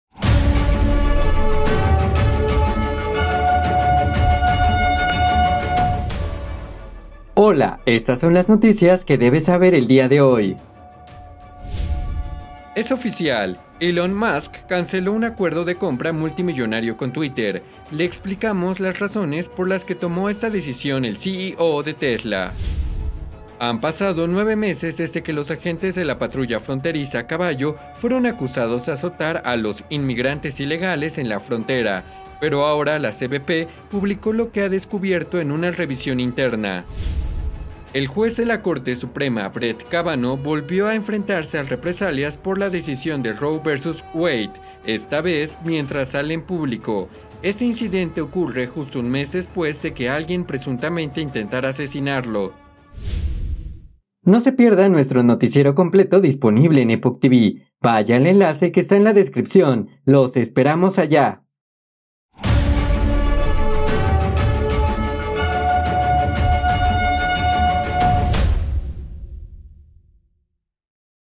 Elon Musk cancela acuerdo con Twitter; CBP: "No hay evidencia" de que agentes azoten a inmigrantes
Noticias de hoy 8 de julio de 2022 en NTD